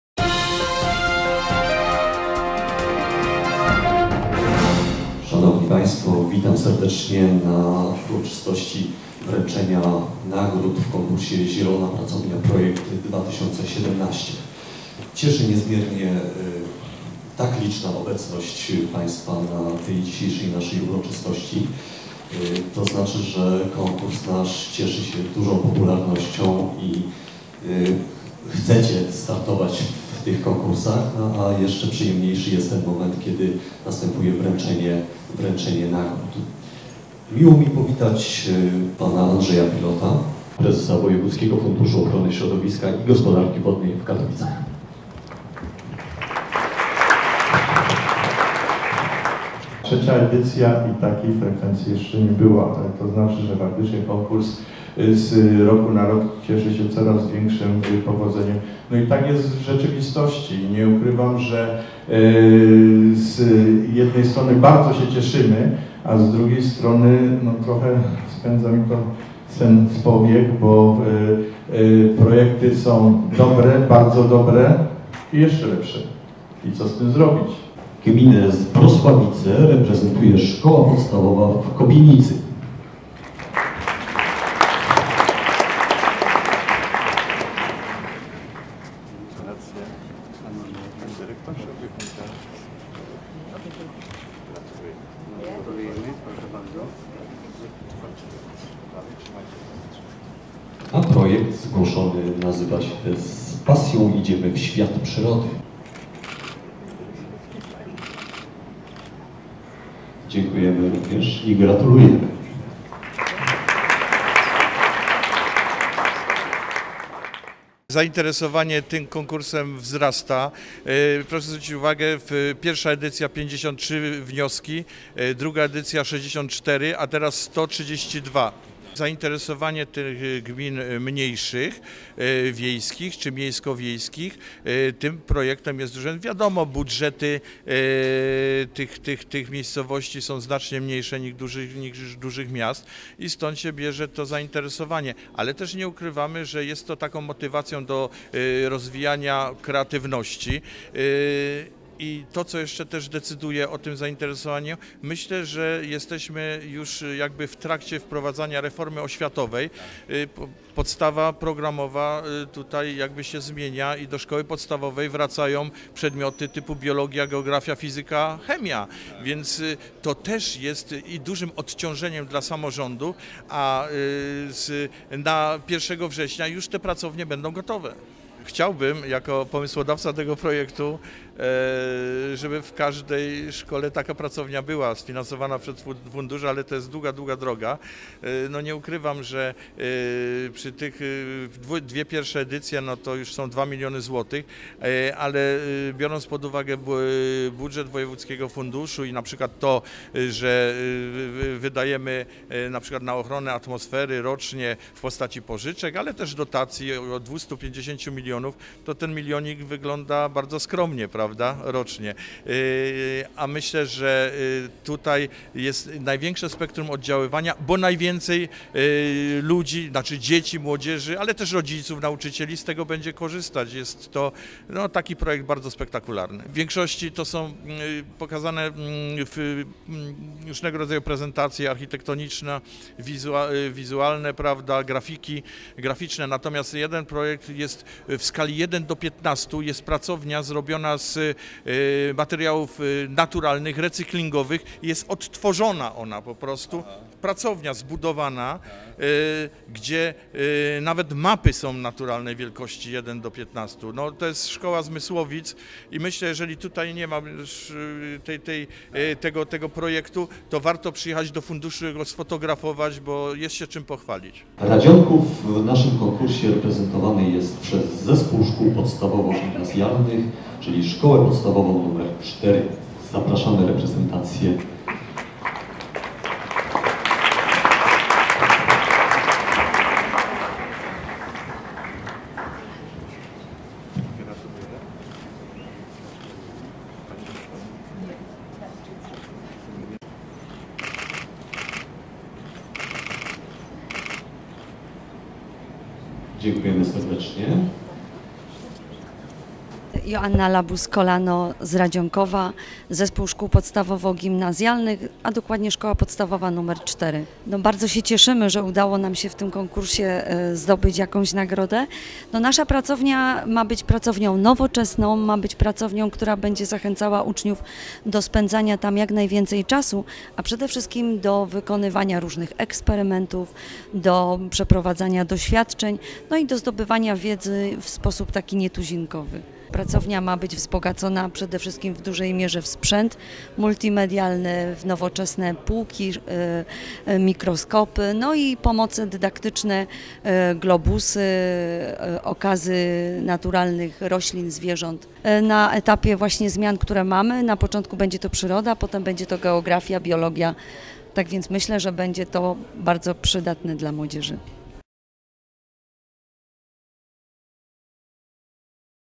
Zielona Pracownia_Projekt 2017
Katowice, 21 kwietnia 2017 r.
Wojewódzki Fundusz Ochrony Środowiska i Gospodarki Wodnej w Katowicach nagrodził laureatów konkursu Zielona Pracownia_Projekt 2017.
Realizacja: Kajetan Berezowski i Jarosław Galusek - nettg.pl